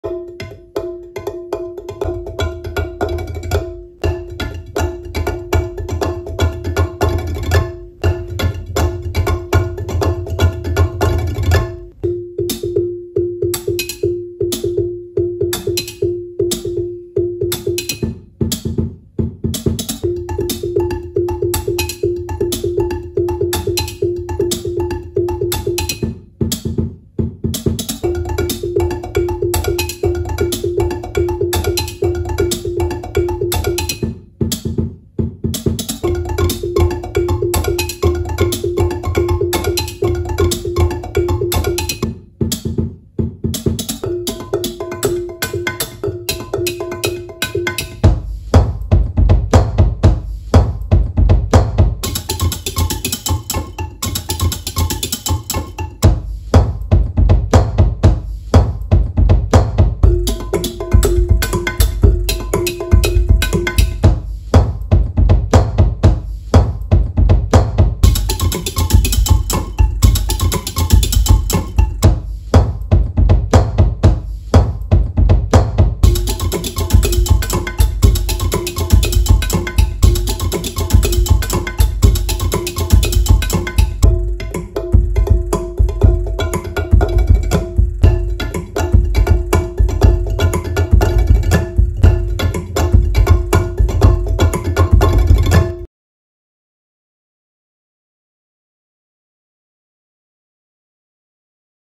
Pots & Pans Beats - Household Percussion/Drumming
Making music with pots & pans. A multi-layered rhythmic song.

I'm a drummer that likes to make beats whilst doing every day activities. Join me on my rhythm adventures.